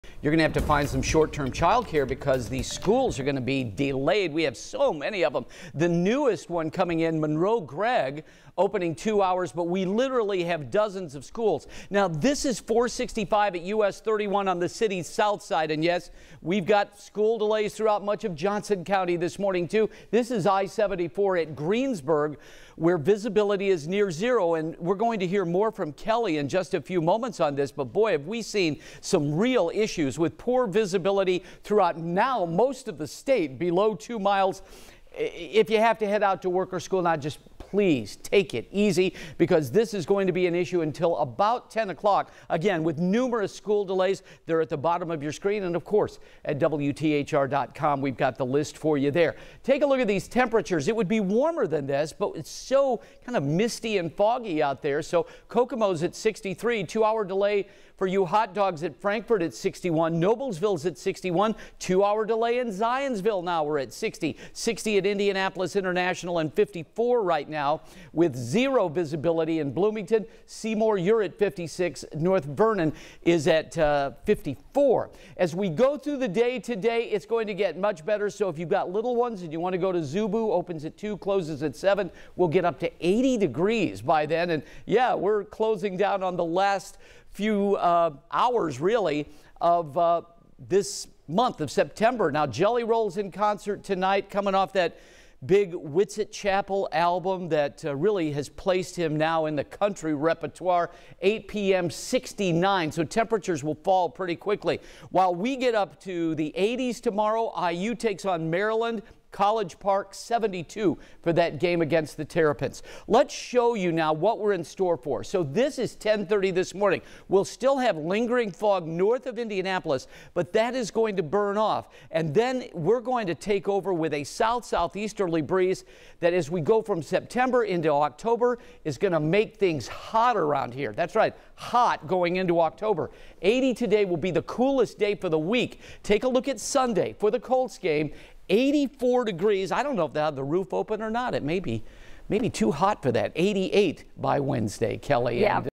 Live Doppler 13 morning forecast - Friday, Sept. 29, 2023
Chuck's morning forecast for Friday, Sept. 29, 2023.